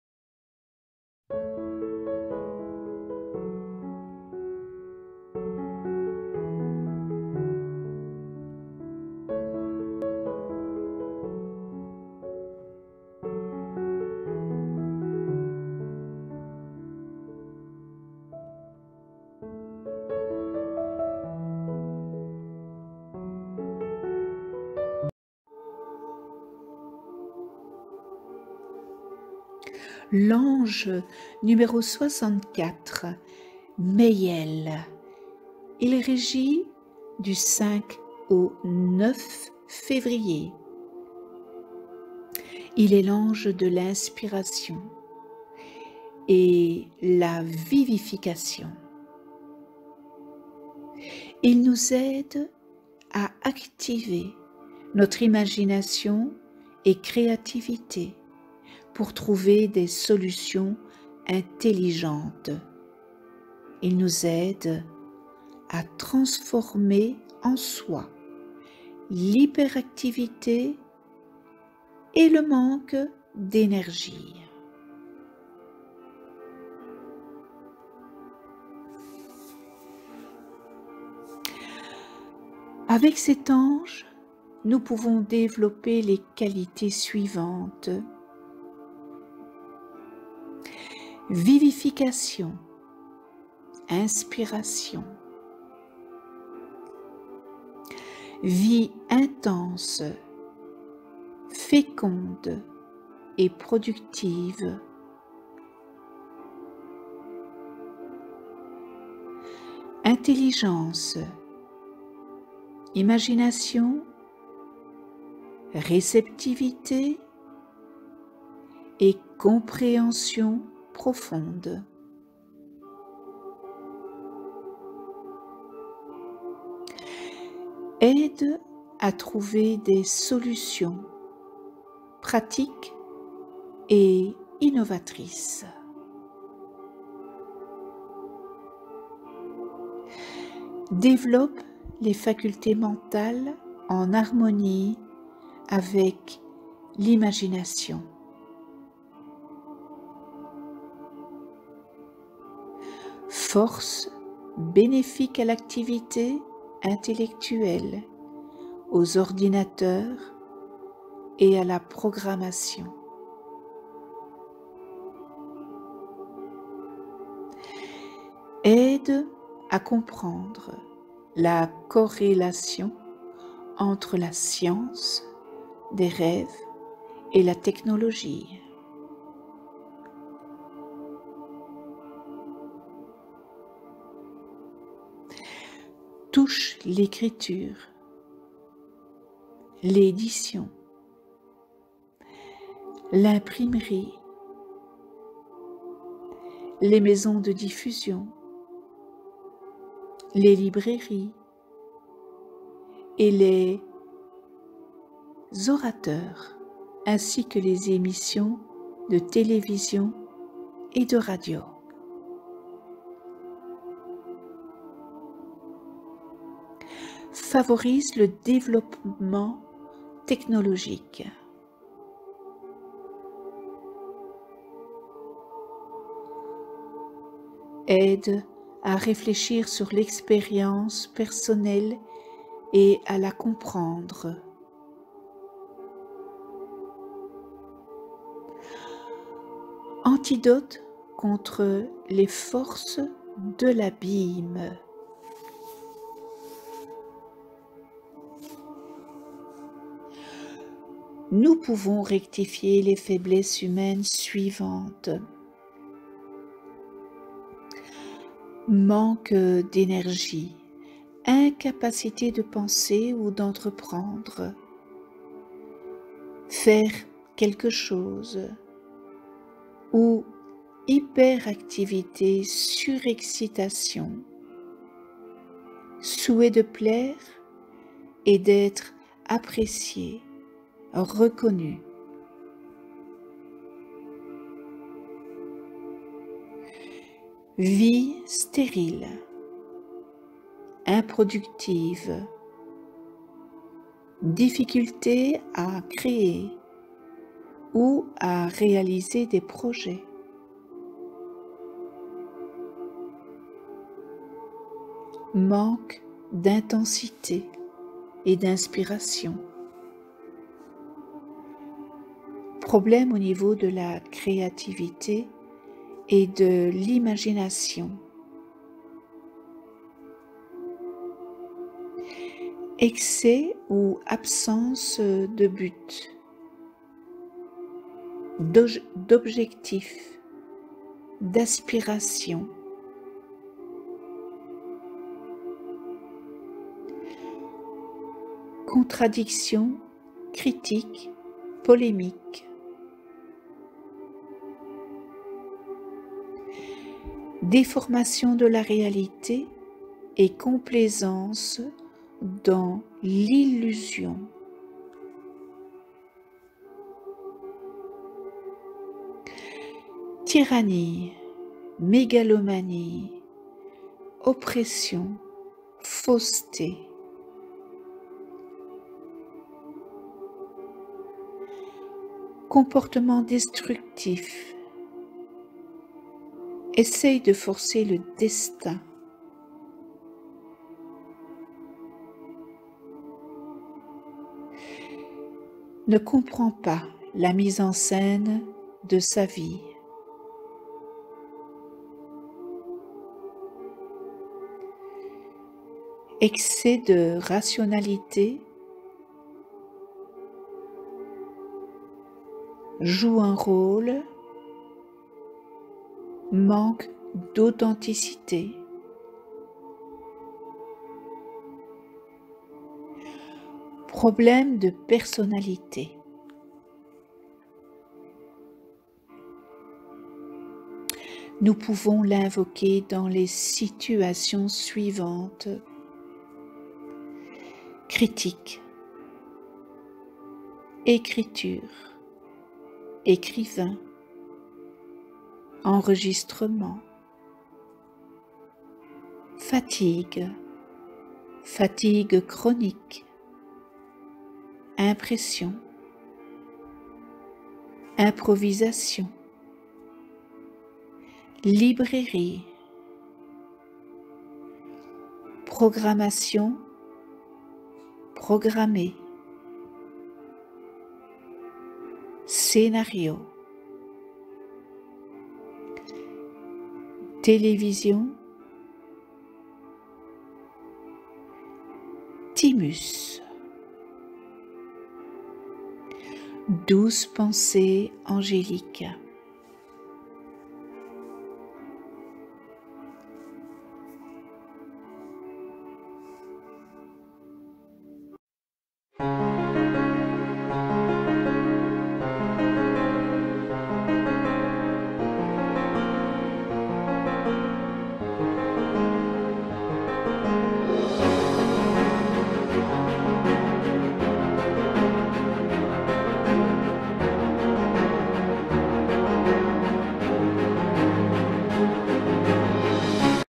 64.Mehiel Qualités / Distorsions Humaines
💚C'est par la force de l'amour que je peux tout transformer💚

Le Calendrier Angélique des 72 Anges avec l'Ange n°64 MEHIEL du 5 au 9 Février 😇 Qualités & Distorsions Humaines

Je vous souhaite un heureux anniversaire à toutes et tous 🎂🎁🎈🎉🌷

Angéliquement 😇😇😇